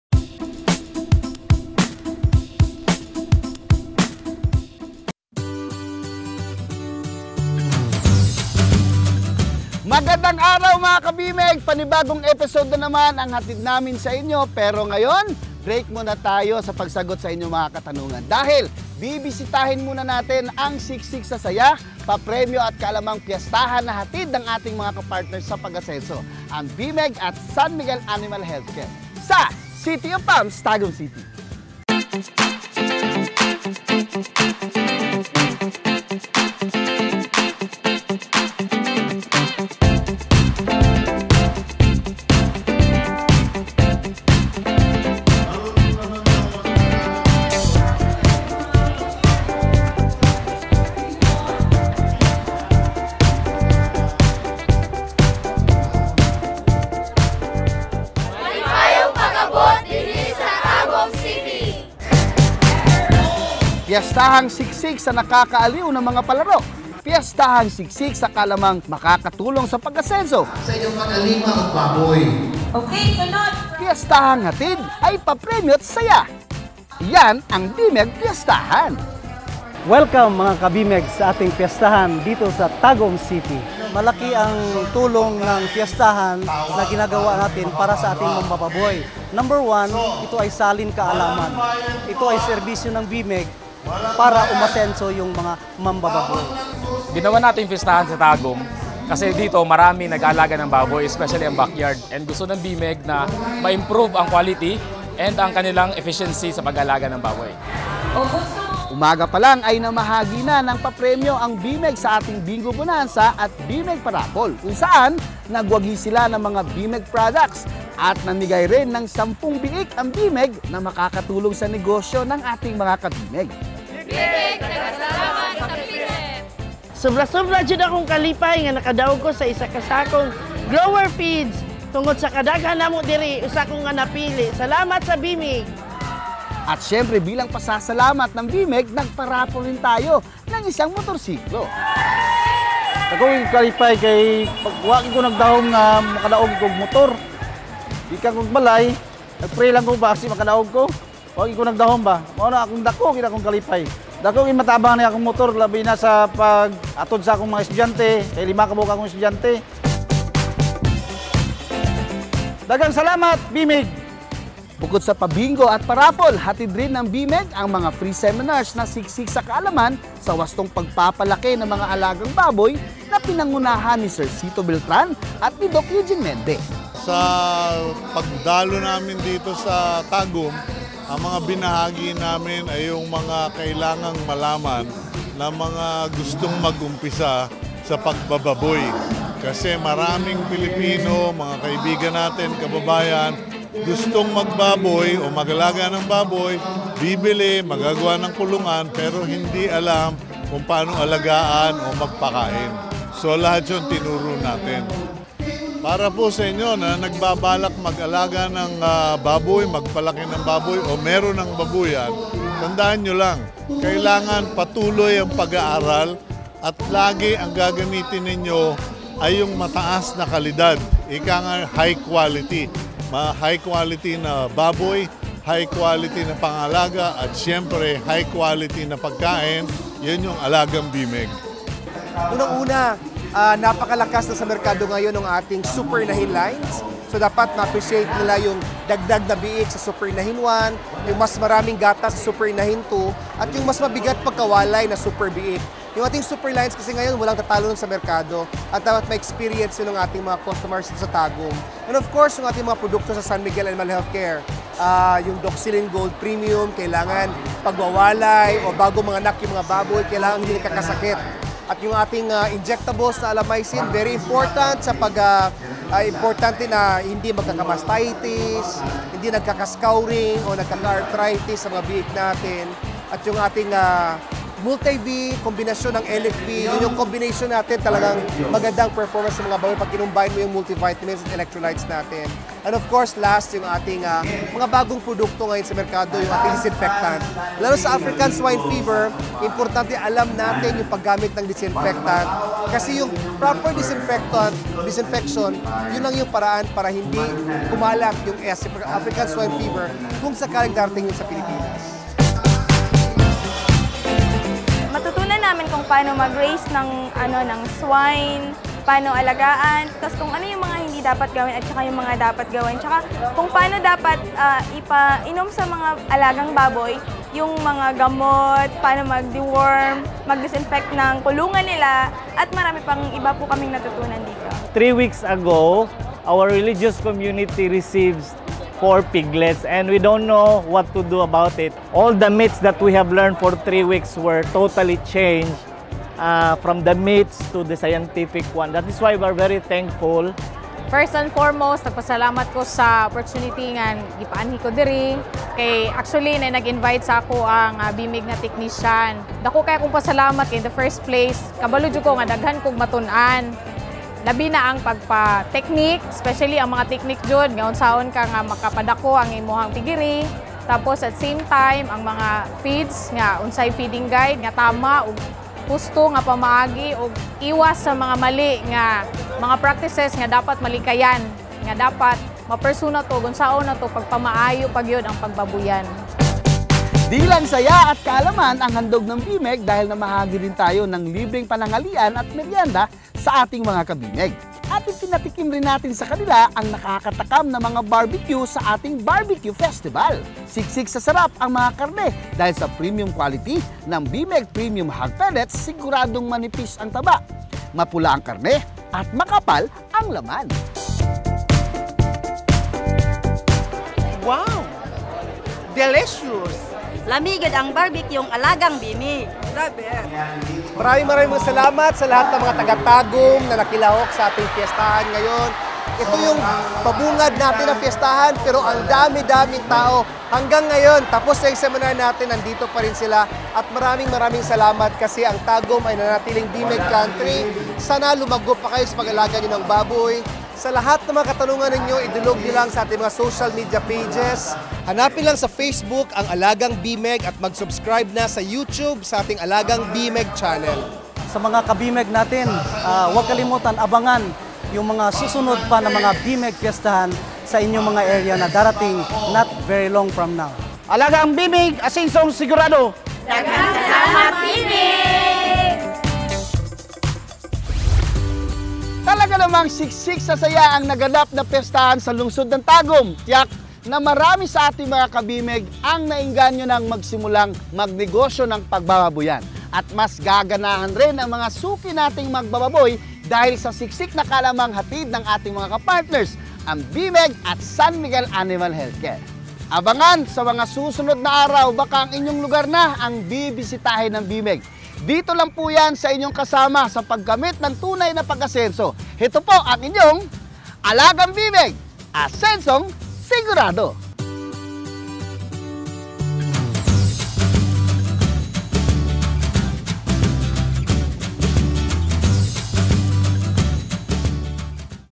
Alagang B-MEG Fiestahan sa Tagum: Libreng Hog Seminar, Papremyo, ATBP!
Tunghayan ang mga kaganapan sa ating pinakaunang LIBRENG HOG SEMINAR sa taong ito: Alagang B-MEG Fiestahan sa Tagum City!

Fiestahang siksik sa saya, papremyo, at kaalaman para sa wastong pag-aalaga ng baboy na hatid ng ating mga ka-partner sa pag-asenso: B-MEG PREMIUM at ng San Miguel Animal Health Care (SMAHC)!

May katanungan ba kayo tungkol sa hog-raising? Mag comment lang sa ibaba ng video na ito para masagot namin sa mga susunod na episode!

Abangan rin ang mga announcement ng susunod na B-MEG Fiestahan sa ating Alagang B-MEG Hog Raising Facebook